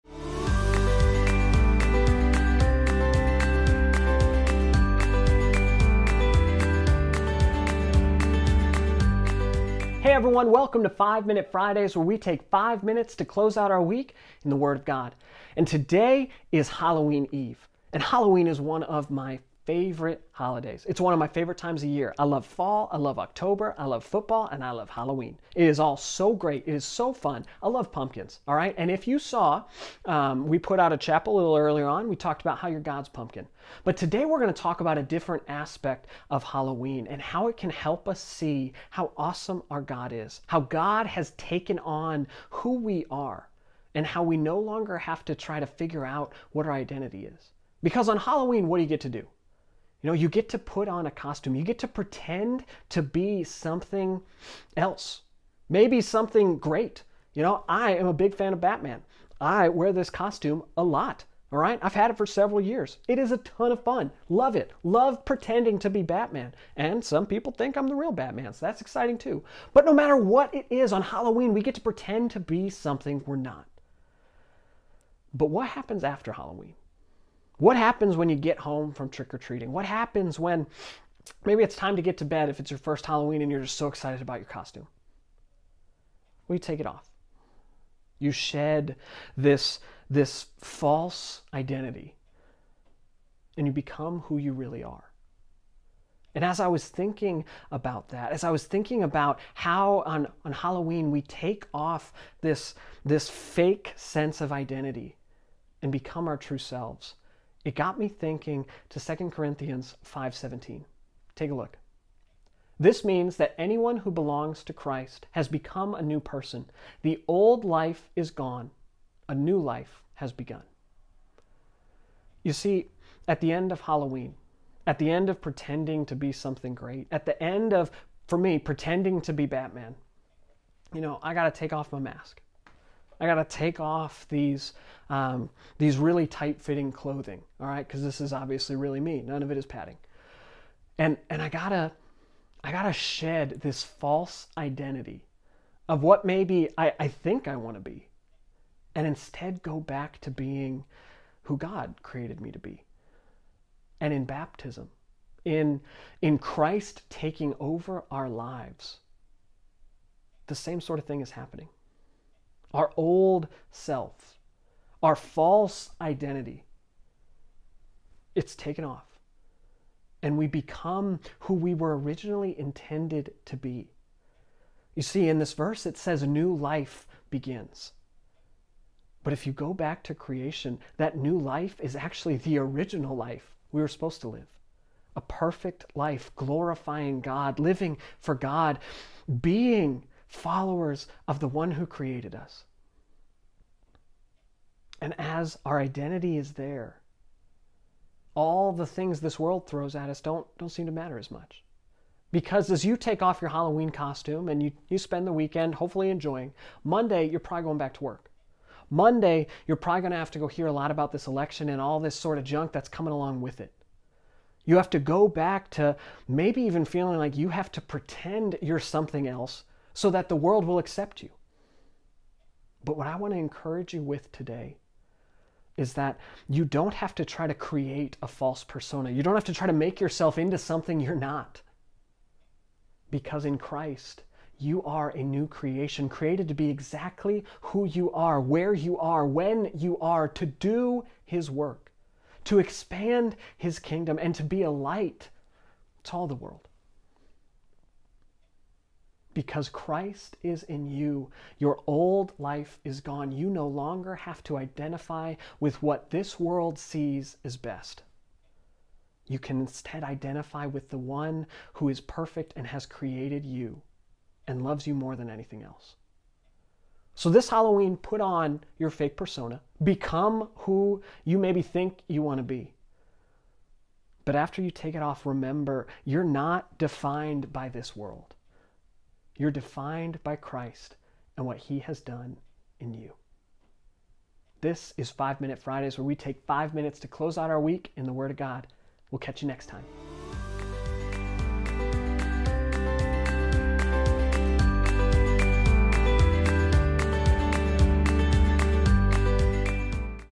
5 Minute Fridays - 2 Corinthians 5:17
Happy Halloween Eve!!! What does Halloween have to do with our relationship God? Watch and see!